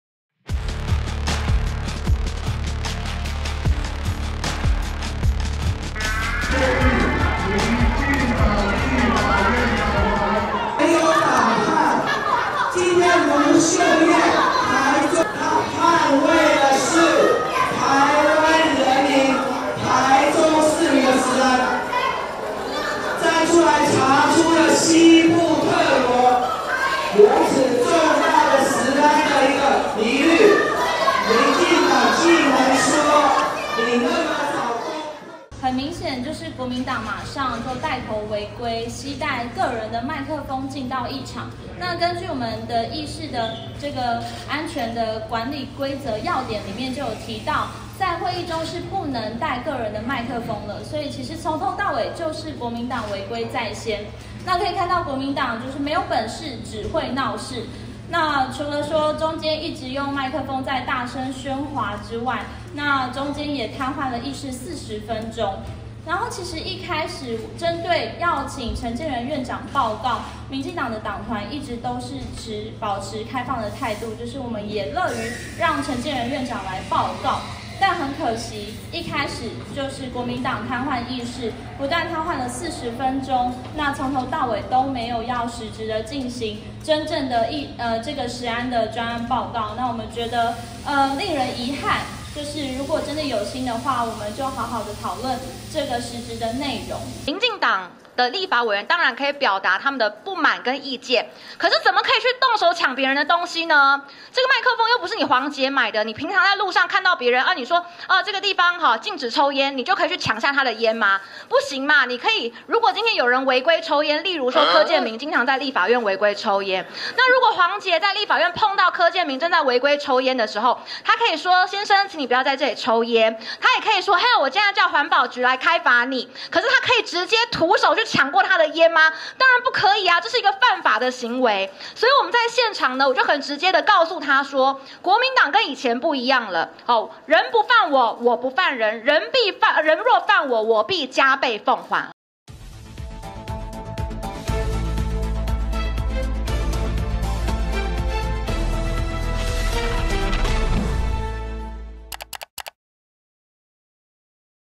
陳建仁報告遭杯葛！藍綠女戰神爆衝突 黃捷、徐巧芯為「搶麥」互槓
記者劉秀敏／台北報導

立法院今（20）日開議，國民黨團原本提案邀請行政院長陳建仁於今日進行食安專案報告，但19日黨團協商時，民眾黨主張明日加開院會，最終協商破局。不過，今天陳建仁進行施政報告前，國民黨團高舉「我們要食安，孩子要健康」等標語杯葛，民進黨立委則拿出「照起工，要開會」等標語反制，立法院長韓國瑜裁示「休息十分鐘」後離場，引發綠營不滿，藍綠雙方也爆發零星推擠衝突，氣氛火爆。